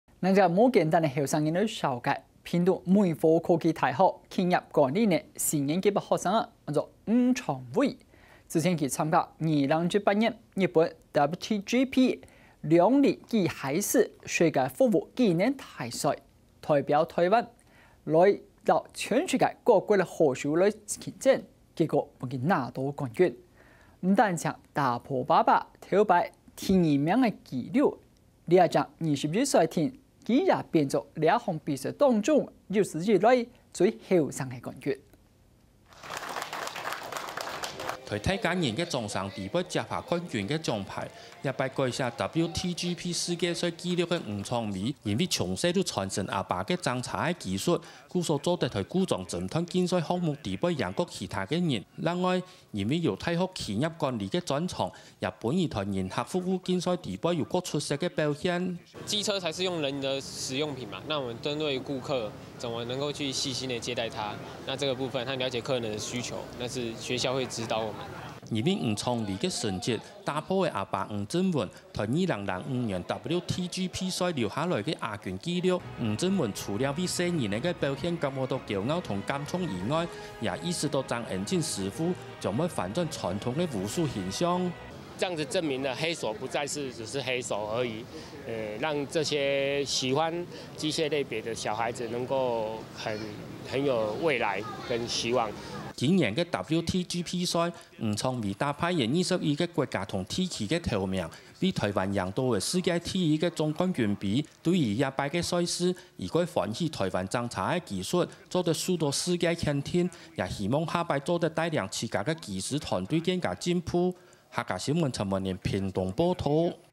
21歲吳昶緯 2018日本WTGP大賽奪冠【客家新聞20181210】
屏東美和科技大學企管系四年級學生吳昶緯，日前參加2018年日本WTGP兩輪機械師世界服務技能大賽，代表台灣，從來自全球各國參賽好手中，奪下冠軍，不僅打破他父親曾寫下的第2名紀錄，現年21歲的他，也成為這項比賽中，歷屆最年輕的冠軍得主。